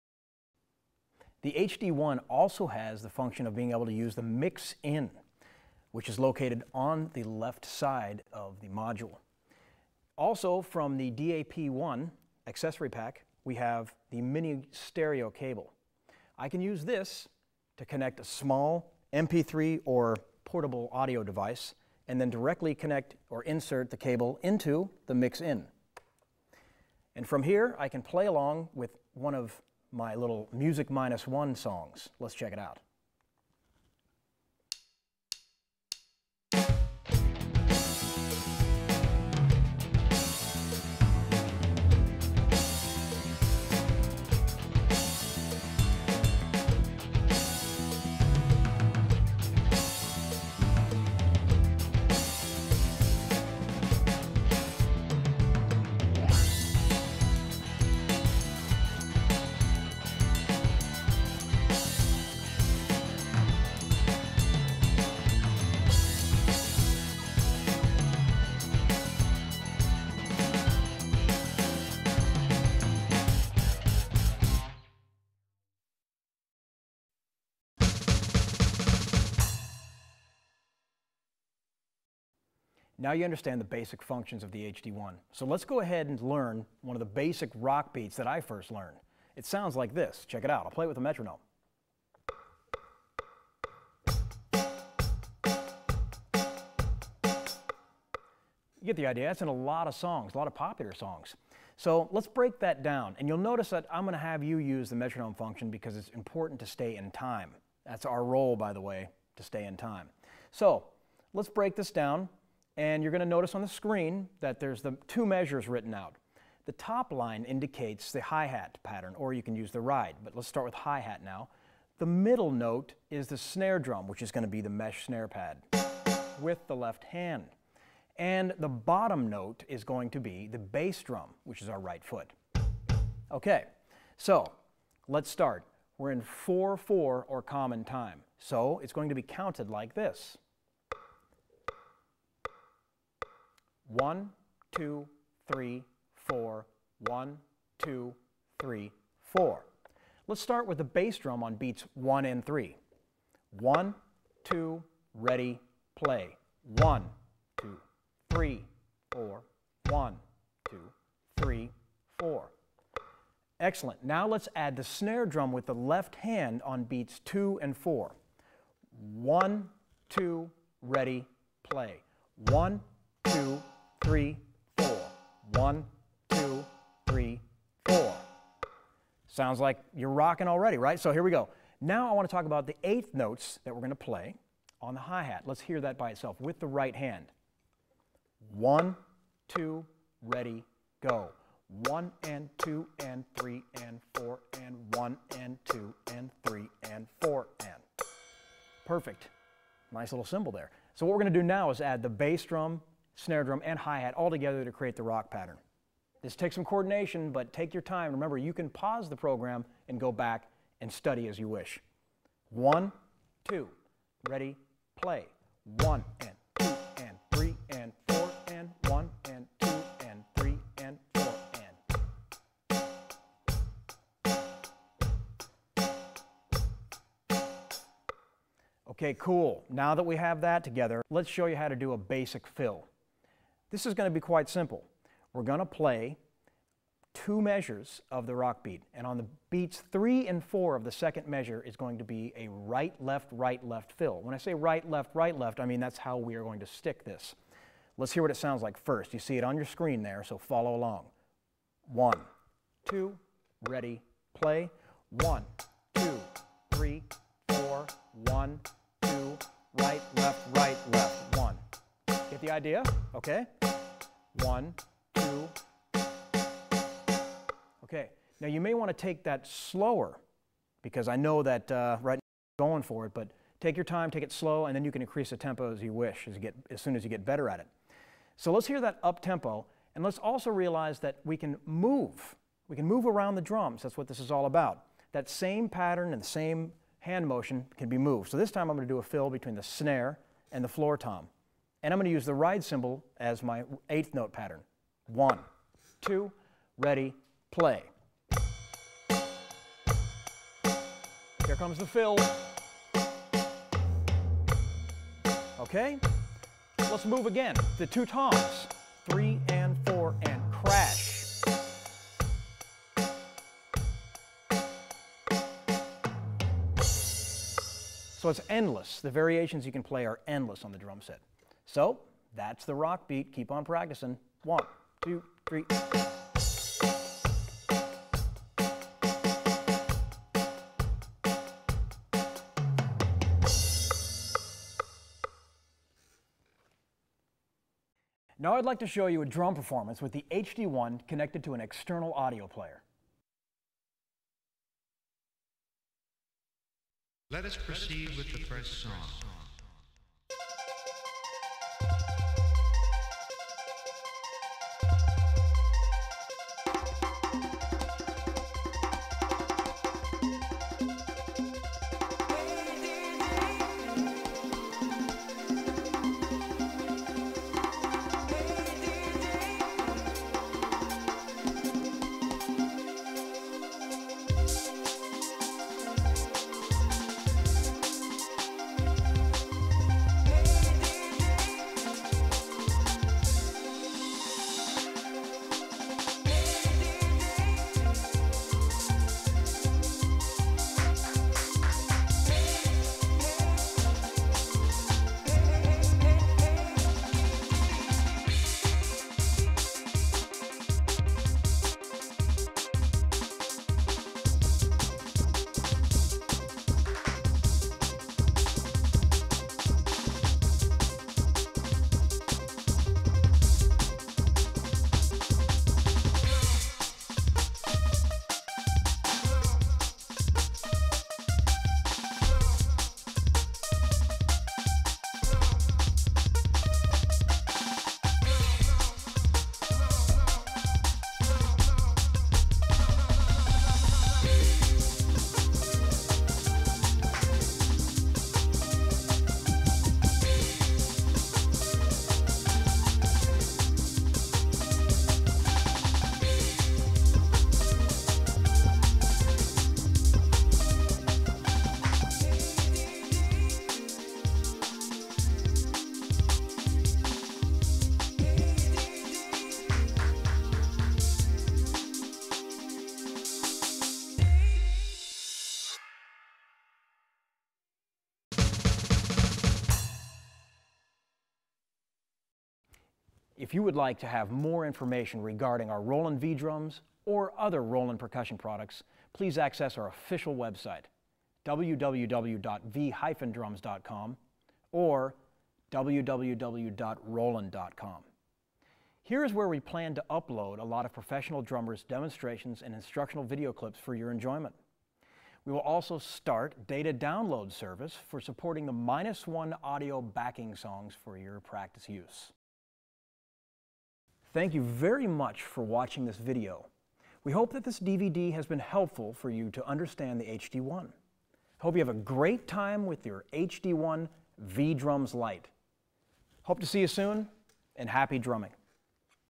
Let`s Try the V-Drums Lite (3/3)
Introducing Roland V-Drums Lite HD-1 by  Johnny Rabb